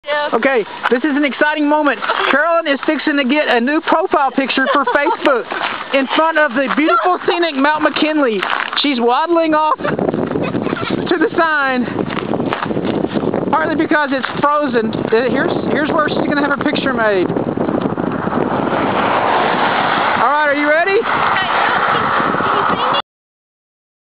Mt. McKinley Facebook profile
Wifey is a photographer, and she gets inspired for a new Facebook profile pic on the road to Mt. McKinley in Alaska.